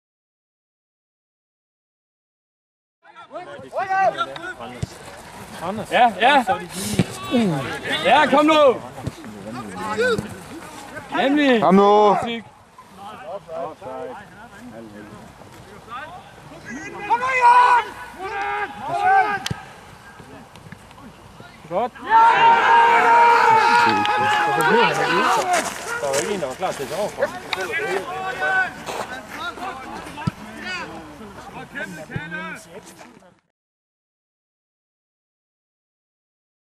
Fremragende mål
Bare lige Kasper Nørgaard og Morten Alstrup der sætter et helt KFA Fodbold hold

- I øvrigt tak til KFA for at filmen kampen